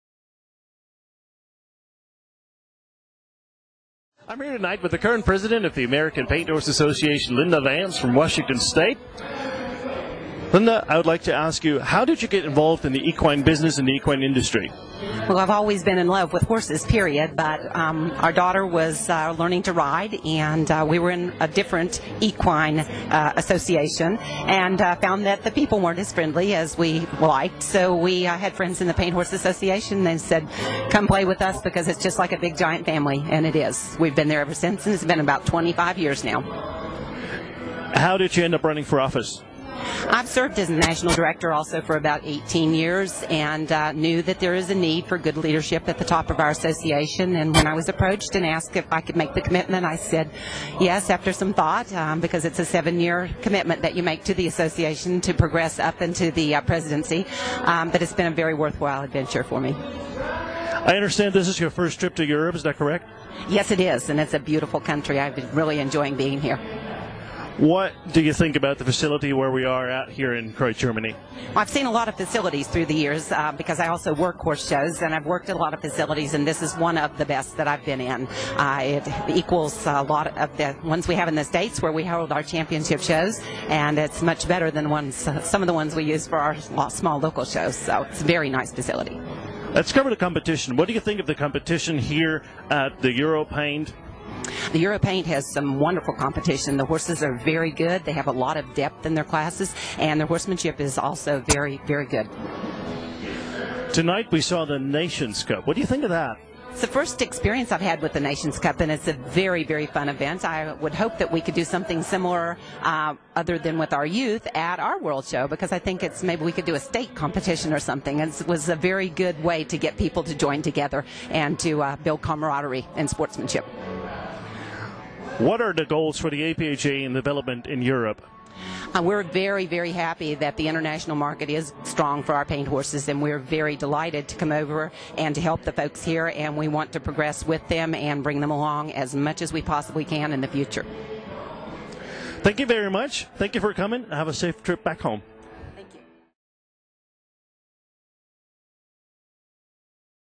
Euro Paint 2010
Interview with 2010 APHA president Linda Vance